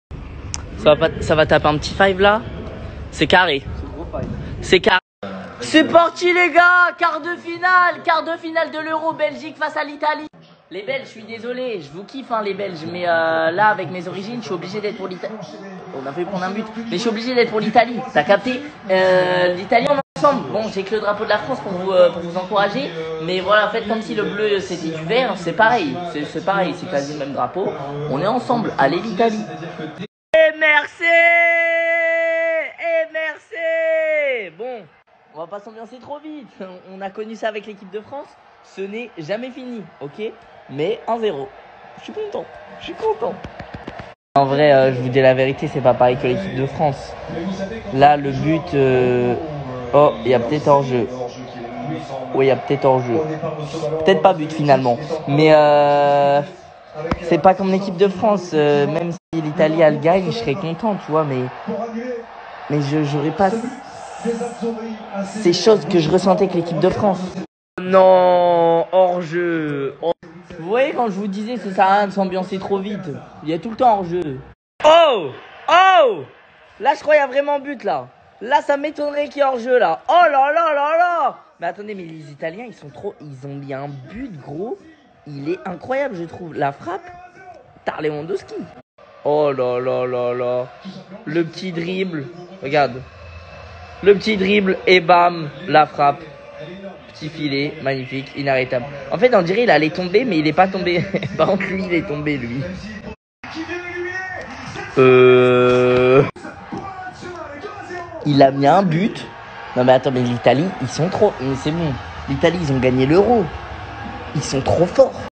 MICHOU RÉAGIT AU MATCH ITALIE 🇮🇹 - BELGIQUE , IL EST CHOQUÉ DE L’ITALIE !
GO TO THE 3000 ABO